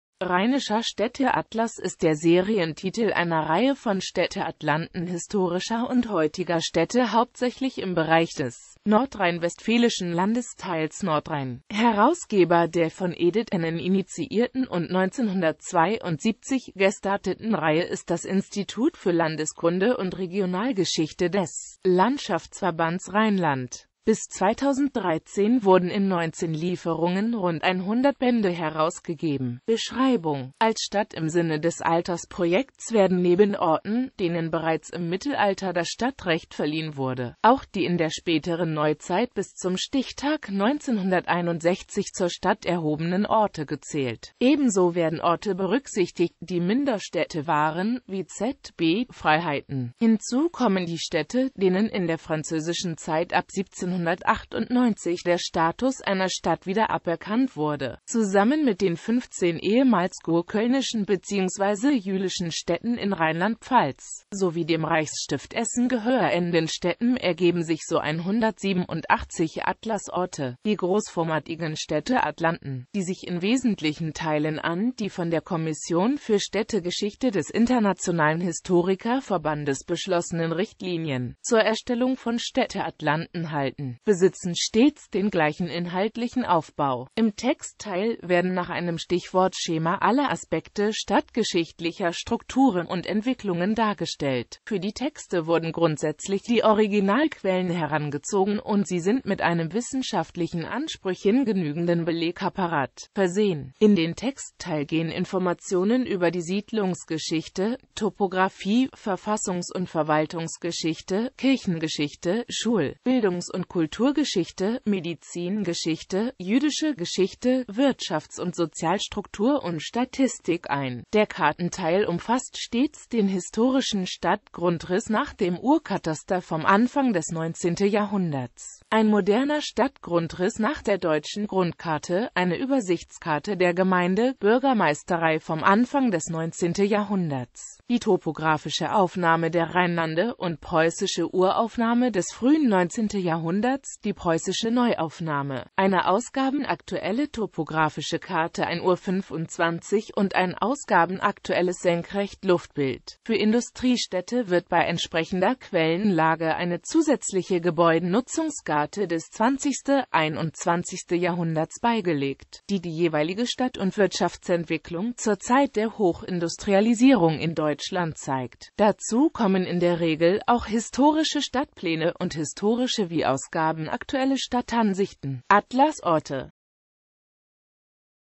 Rheinischer Städteatlas
Vielen Dank für Ihre Unterstützung:

Rheinischer Städteatlas ist der Serientitel einer Reihe von Städteatlanten historischer und heutiger Städte hauptsächlich im Bereich des nordrhein-westfälischen Landesteils Nordrhein.Herausgeber der von Edith Ennen initiierten und 1972 gestarteten Reihe ist das Institut für Landeskunde und Regionalgeschichte des Landschaftsverbands Rheinland.Bis 2013 wurden in 19 Lieferungen rund 100 Bände herausgegeben.